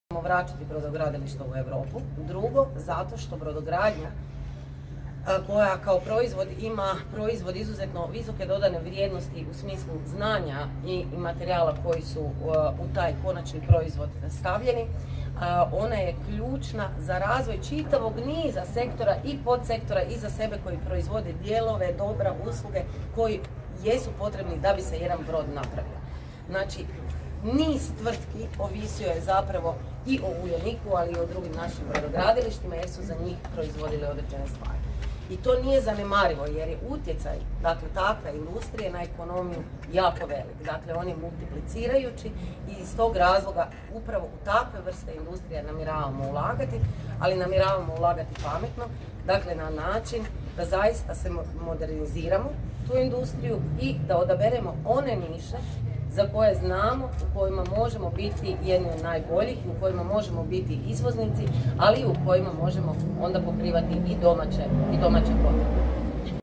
Sandra Benčić: Vraćati ćemo brodogradilišta u Europu
Na Poljani predstavnici stranke Možemo dijelili su reklamni materijal pozivajući građane da svoj glas daju njima objašnjavajući i razloge. Šibenski predstavnici na listi su Ana Šimić, Ivan Slavica i Maja Sintić